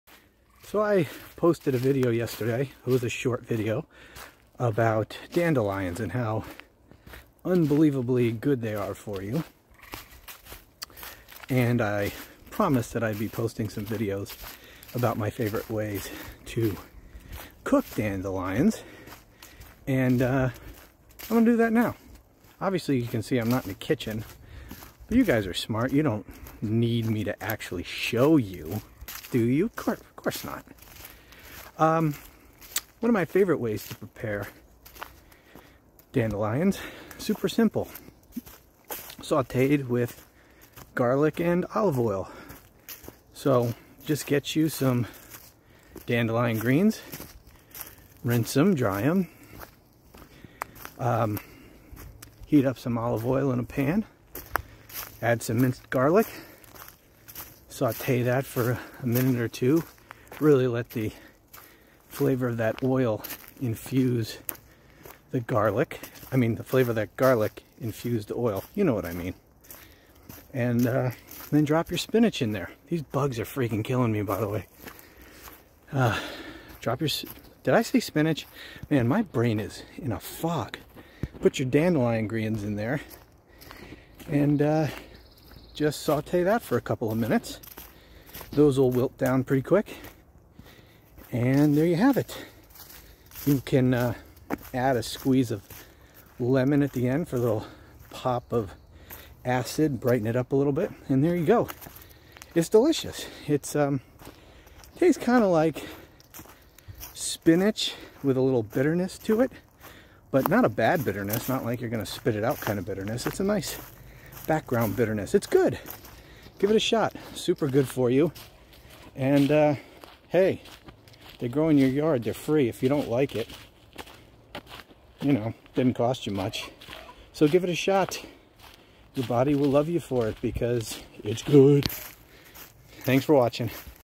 How to Cook Dandelions
Dandelions are not just weeds growing in your yard. They are great for eating. Packed with vitamins, minerals, and antioxidants, dandelions are great for your health. But how do you cook dandelions? There are many ways to cook and eat dandelions, but this is one of my favorites. Follow this simple recipe for dandelions with garlic and oil.